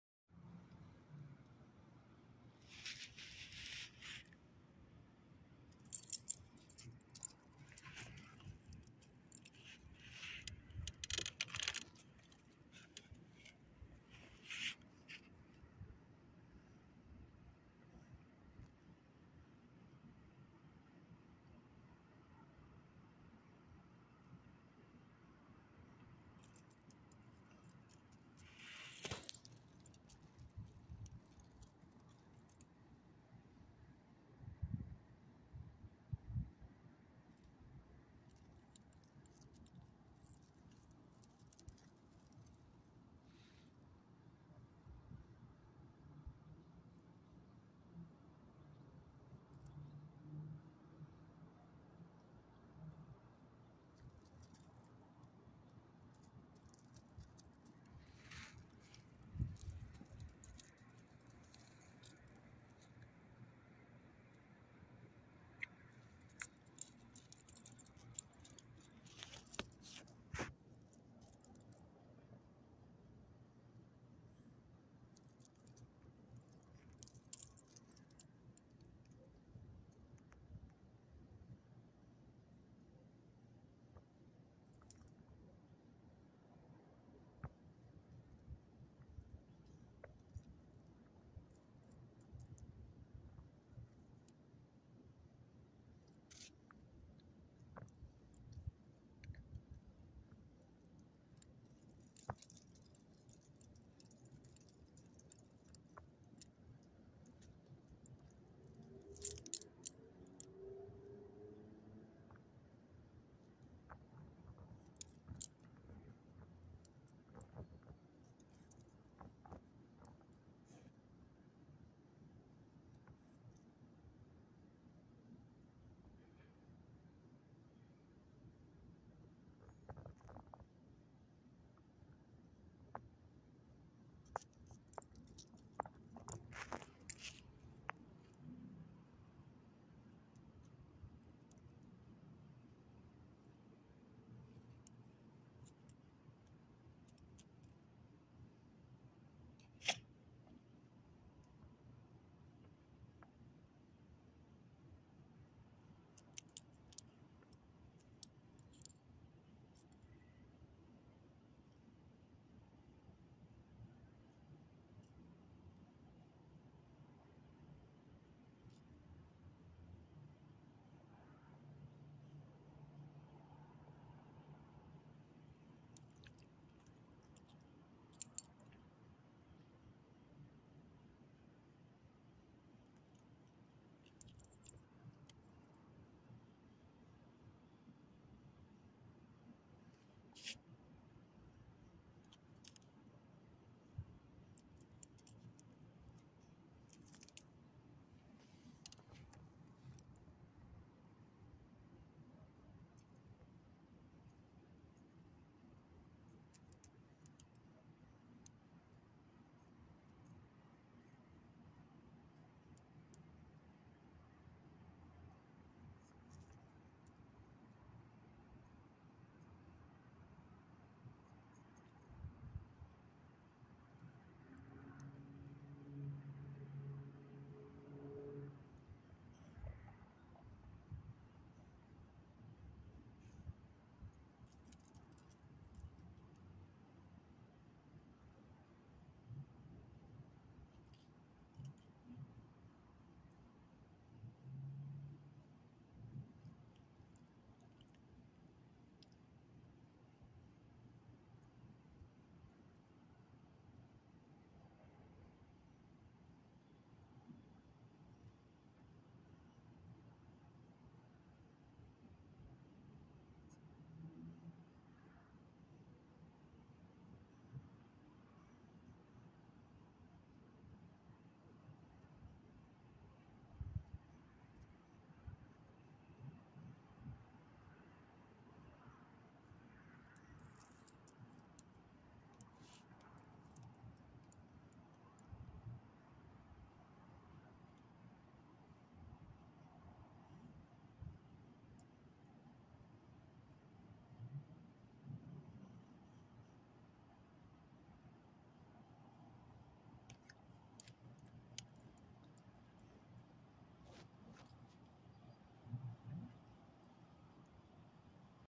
Ozzyta's tail's low a long time, maybe she's bored(glancing off some) but at least she kinda calm975
I trimmed Ozzyta's nail before our walk. She had a couple nails that needed clipping. I prefer to clip her nails only a small bit, more often, so I don't cut too much & make a single nail bleed by accident if I have cut too much. She did appear more relaxed after I'd clipped her nails. She didn't walk faster, but seemed happier.